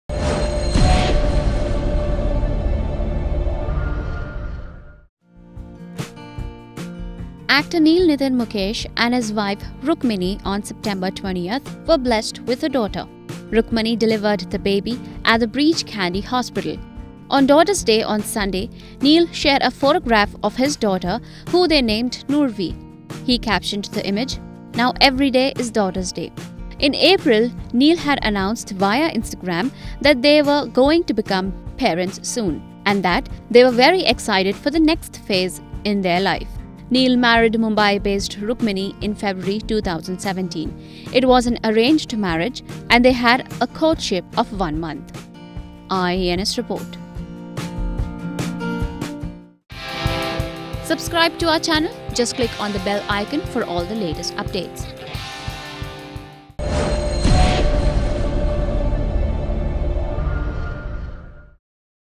Neil Nitin Mukesh shares daughter's First Pic
Actor Neil Nitin Mukesh and his wife Rukmini on September 20th were blessed with a daughter. Rukmini delivered the newborn at the Breach Candy Hospital around 3:30 p.m., a source told IANS. On 'Daughter's Day' on Sunday, Neil shared a photograph of his daughter, who they named 'Nurvi'.